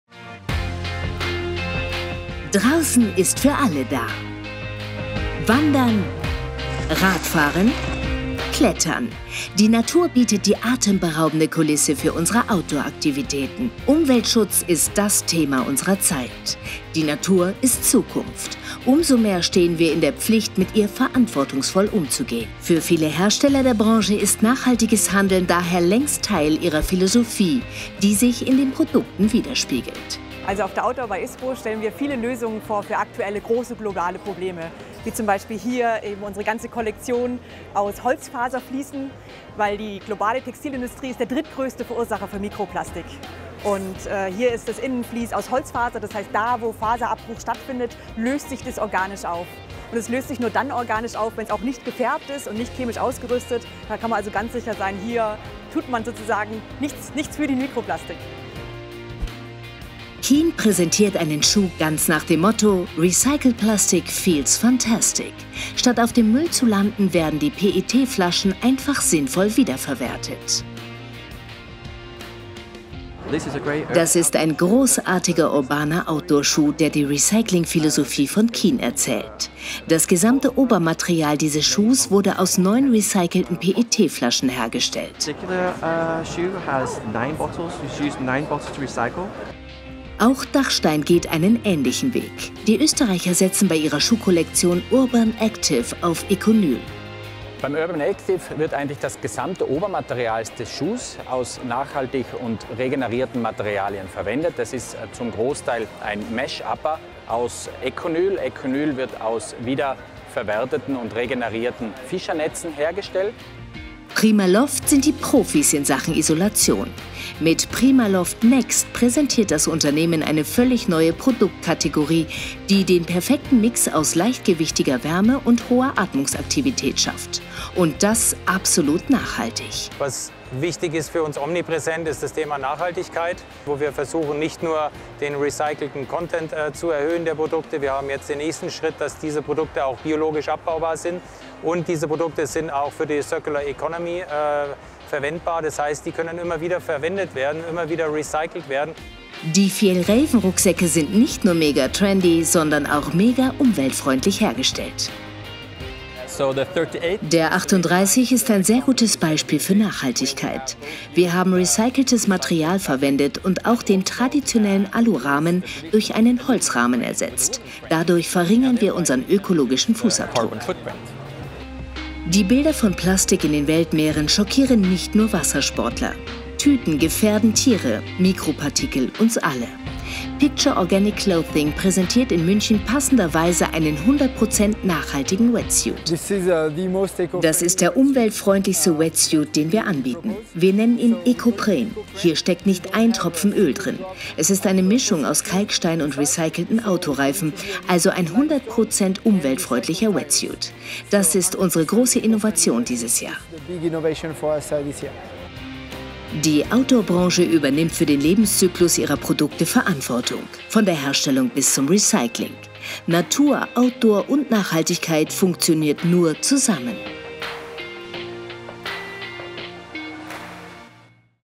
OutDoor by ISPO | Nachhaltigkeit im Outdoorbereich
Draußen ist für alle da! Wandern, Radfahren, Klettern: Die Natur bietet die atemberaubende Kulisse für unsere Outdooraktivitäten. Umweltschutz ist DAS Thema unserer Zeit. Die Natur ist Zukunft. Umso mehr stehen wir in der Pflicht mit ihr verantwortungsvoll umzugehen. Für viele Hersteller der Branche ist nachhaltiges Handeln daher längst Teil ihrer Philosophie, die sich in den Produkten widerspiegelt.

„Also auf der OutDoor by ISPO stellen wir viele Lösungen vor für aktuelle, große globale Probleme. Wie zum Beispiel hier unsere ganze Kollektion aus Holzfaserfleece, weil die globale Textilindustrie der drittgrößte Verursacher von Mikroplastik ist. Und hier ist das Innenflies aus Holzfaser. Da wo Faserabrieb stattfindet löst sich das organisch auf. Und das löst sich nur dann organisch auf, wenn es nicht gefärbt ist und nicht chemisch ausgerüstet, dann kann man sicher sein: Hier tut man nichts für Mikroplastik.“

Keen präsentiert einen Schuh ganz nach dem Motto: Recycled Plastic – Feels Fantastic. Statt auf dem Müll zu landen, werden die PET Flaschen einfach sinnvoll wiederverwertet.

“Das ist ein großartiger urbaner Outdoor-Schuh, der die Recyclingphilosophie von Keen erzählt. Das gesamte Obermaterial dieses Schuhs wurde aus neun recycelten PET Flaschen hergestellt.“

Auch Dachstein geht einen ähnlichen Weg. Die Österreicher setzen bei ihrer Schuhkollektion Urban Active auf Econyl.

„Beim Urban Active wird das gesamte Obermaterial des Schuhs aus nachhaltig und regenerierten Materialien verwendet. Das ist zum Großteil ein Mesh Upper aus Econyl. Econyl wird aus wiederverwerteten und regenerierten Fischernetzen hergestellt.“

Primaloft sind die Profis in Sachen Isolation. Mit PrimaLoft® NEXT präsentiert das Unternehmen eine völlig neue Produkt-Kategorie, die den perfekten Mix aus leichtgewichtiger Wärme und hoher Atmungsaktivität schafft. Und das absolut nachhaltig.

„Was wichtig für uns ist, ist das Thema Nachhaltigkeit, wo wir versuchen nicht nur den recycelten Content der Produkte zu erhöhen, wir haben jetzt den nächsten Schritt, dass diese Produkte auch biologisch abbaubar sind. Und diese Produkte sind auch für die Circular Economy verwendbar. Das heißt, die können immer wieder verwendet werden, immer wieder recycelt werden.“

Die Fjällräven Rucksäcke sind nicht nur mega trendy, sondern auch mega umweltfreundlich hergestellt.

„Der 38 ist ein sehr gutes Beispiel für Nachhaltigkeit. Wir haben recyceltes Material verwendet und auch den traditionellen Alurahmen durch einen Holzrahmen ersetzt. Dadurch verringern wir unseren ökologischen Fußabdruck.“

Die Bilder von Plastik in den Weltmeeren schockieren nicht nur Wassersportler. Tüten gefährden Tiere, Mikropartikel uns alle. Picture Organic Clothing präsentiert in München passender Weise einen 100 Prozent nachhaltigen Wetsuit.

„Das ist der umweltfreundlichste Wetsuit den wir anbieten. Wir nennen ihn Ecopren. Hier steckt nicht ein Tropfen Öl drin. Es ist eine Mischung aus Kalkstein und recycelten Autoreifen, also ein 100 prozentig umweltfreundlicher Wetsuit. Das ist unsere große Innovation dieses Jahr.“

Die Outdoor-Branche übernimmt für den Lebenszyklus ihrer Produkte Verantwortung. Von der Herstellung bis zum Recycling. Natur, Outdoor, und Nachhaltigkeit funktioniert nur zusammen.